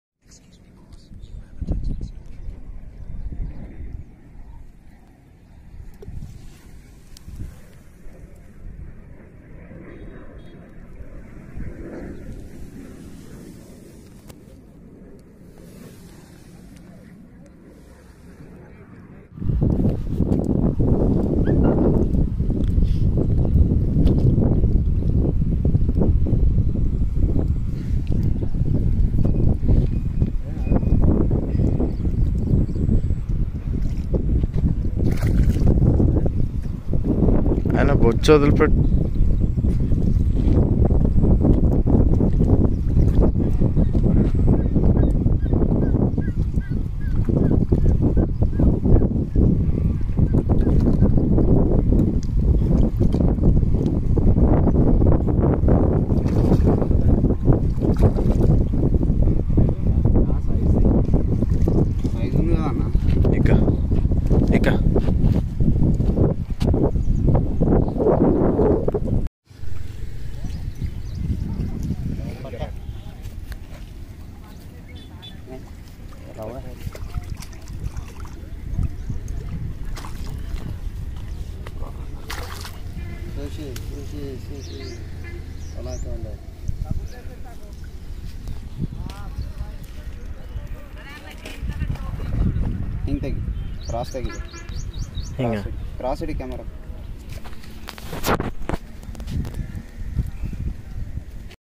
Amazing ||Catla and Rohu Fishing||Best fishing..........
Another video from power star fishing,
We caught another catla and rohu fish and
And played with singh ka shikkar fishing with
Tripel hook .,
Bait used that konda mean rice polish, and wheat flour mix it well and play it, and it will be successful bait for rohu and mirgal fish's.

// Support//.